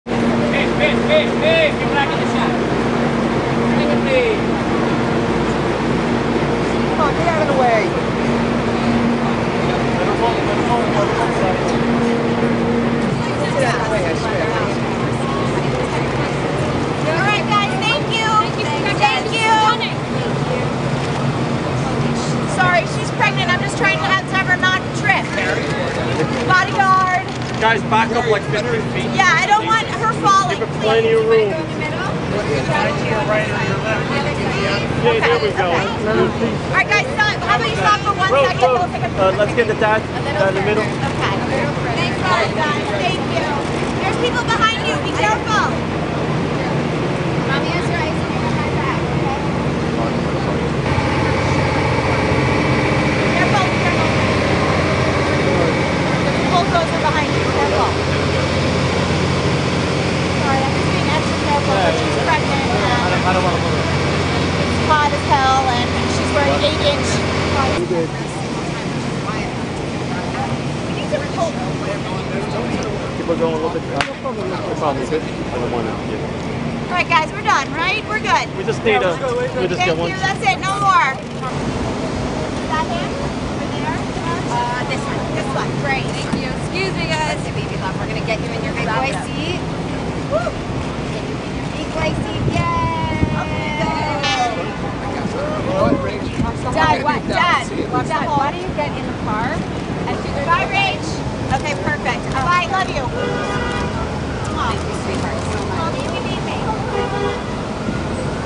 Rachel Zoe with her son Skyler and her husband Rodger at Lincoln Center in New York City
We spotted Fashion designer Rachel Zoe, Skyler Morrison Berman, and Rodger Berman leave the Mercedes-Benz Fashion Week at Lincoln Center. Her assistant repeat many times that photographers should be really carefull as Rachel is pregnant

New York City, NY, USA on Wednesday September 11, 2013